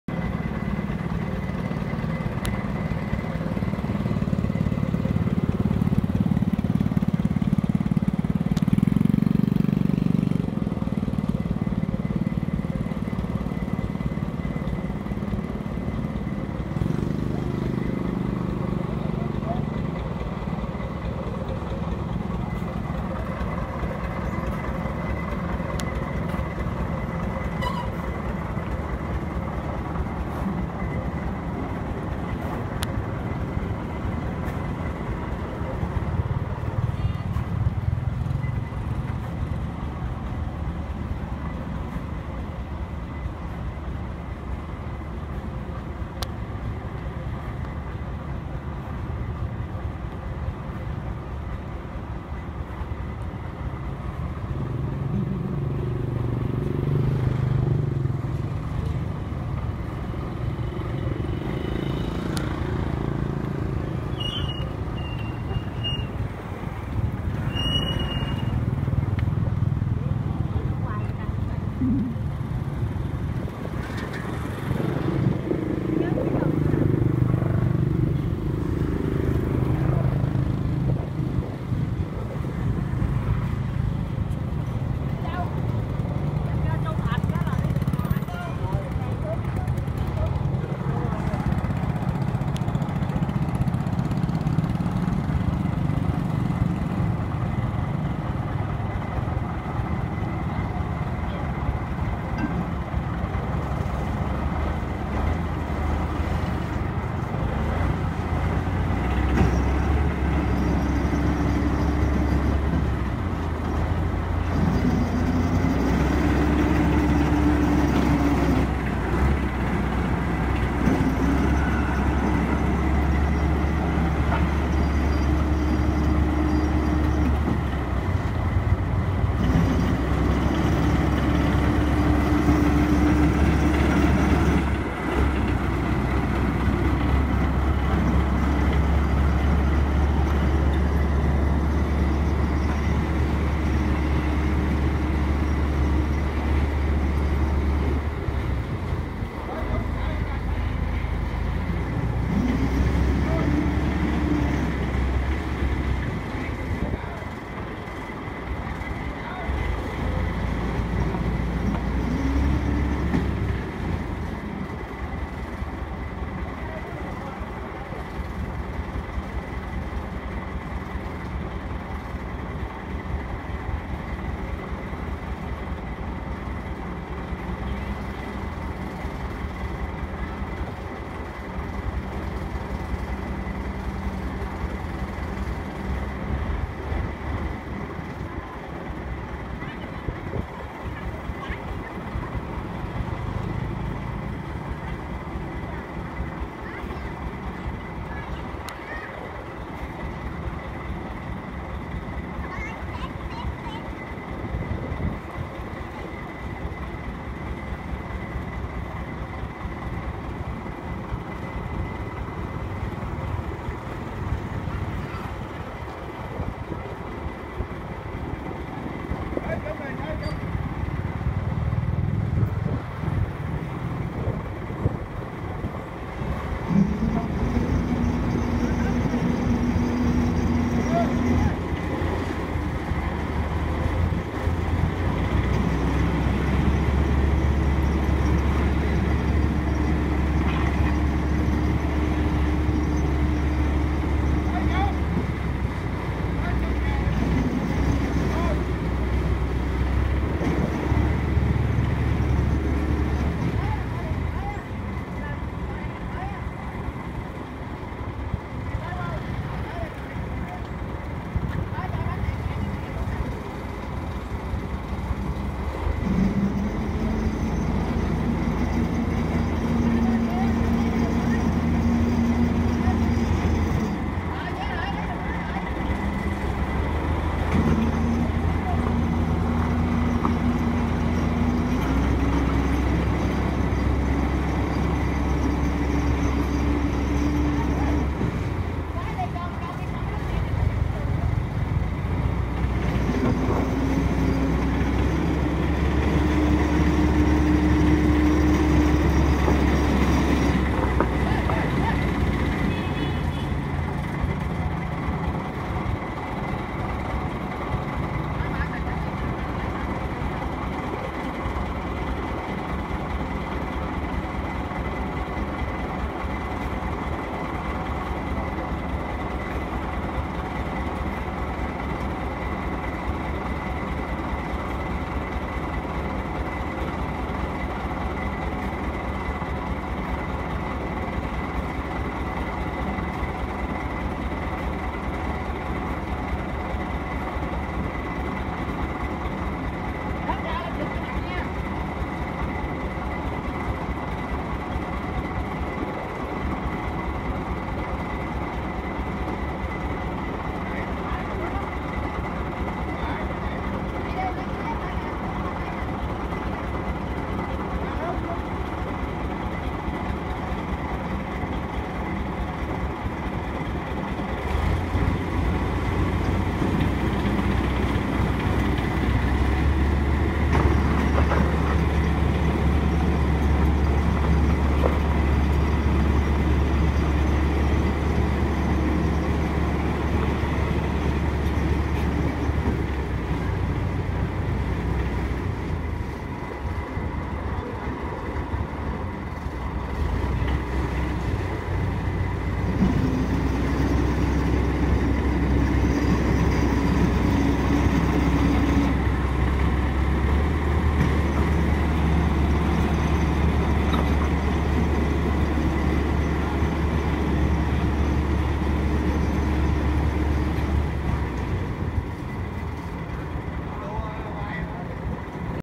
Đường Mới Đang Thi Công Cho Bến Phà Mương Ranh - Phía Phà - Phần 2 - Xe Lu Mắc Kẹt
Quay gần Chợ Châu Thành
2022.08:25 10:14
Phạm vi công cộng